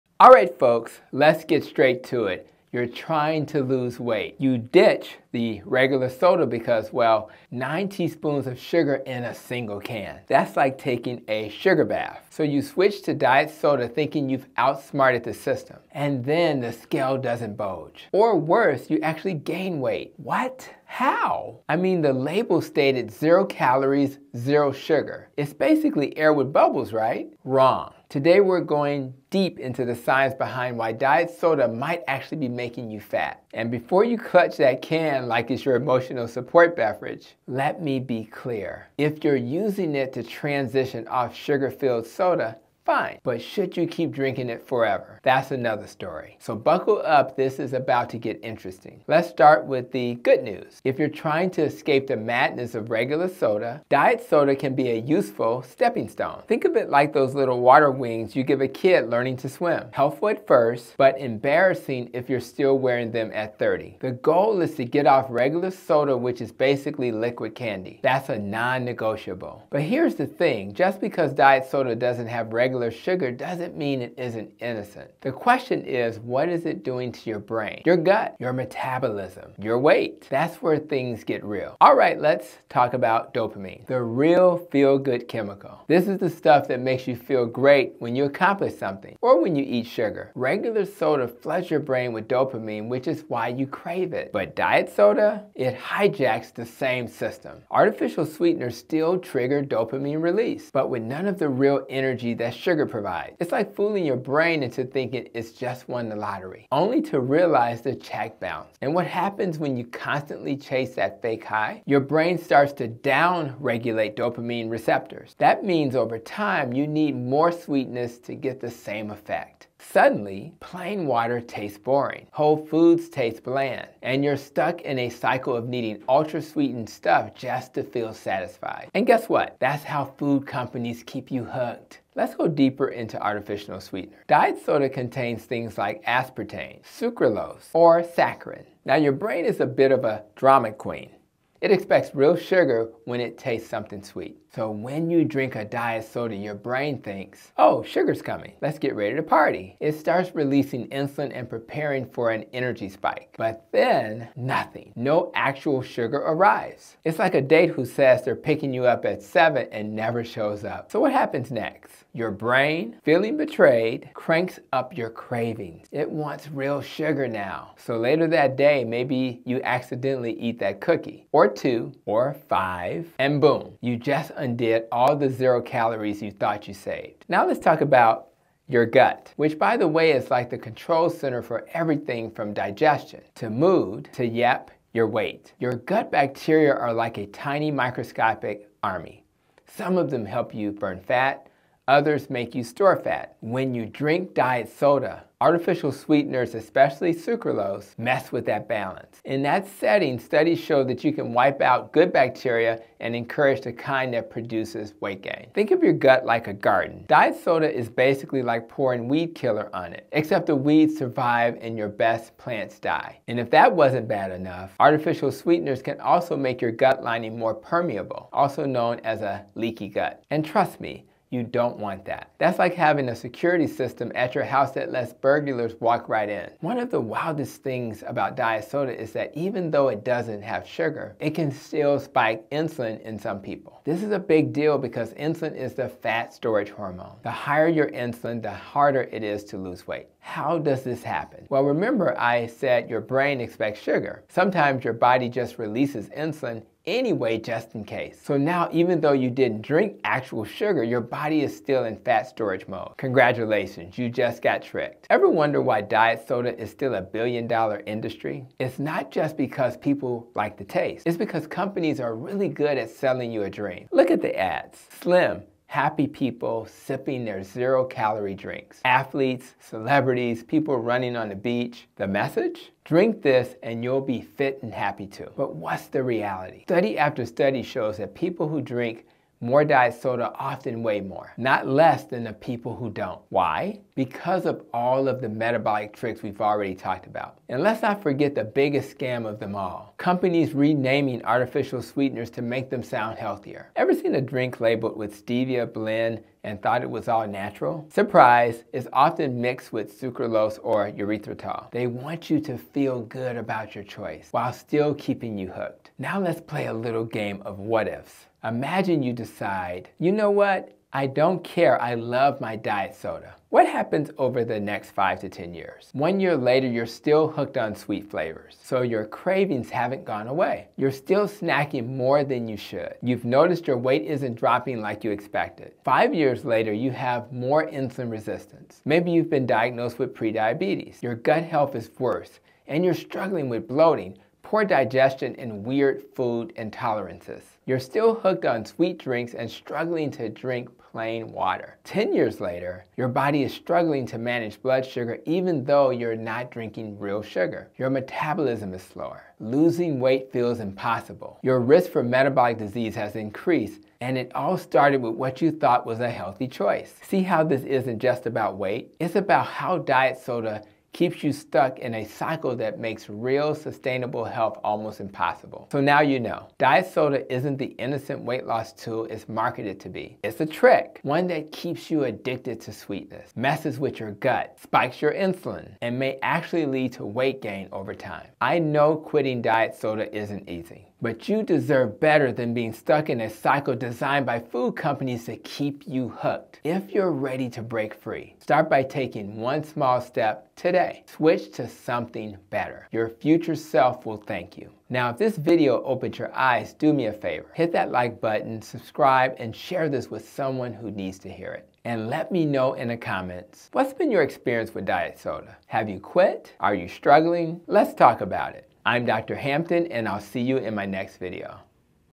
The Diet Soda Lie
Think diet soda is helping you lose weight? Think again! In this video, I break down the shocking truth about how diet soda can actually lead to weight gain, cravings, insulin spikes, and gut health issues—even though it has zero sugar.

🚨 If you’re using diet soda to transition off regular soda, that’s one thing. But should you keep drinking it? The science says NO. I’ll explain:
✔️ How diet soda tricks your brain into eating more
✔️ Why artificial sweeteners still spike insulin and store fat
✔️ The gut-damaging effects of diet soda
✔️ How food companies keep you addicted
✔️ What happens if you drink diet soda for 10 years

👉 If you want real, lasting weight loss and metabolic health, it’s time to rethink diet soda. Watch until the end for my top healthy alternatives and let’s get you on track!

Or are you struggling to kick the habit? Let’s talk about it!

Video Timeline (One Line Per Topic)
00:00 Introduction
01:22 The dopamine trap: How diet soda hijacks your brain
02:58 The gut microbiome disaster—how diet soda messes with your digestion
04:15 Industry and diet soda
05:13 What happens over 5-10 years if you don't stop consuming diet soda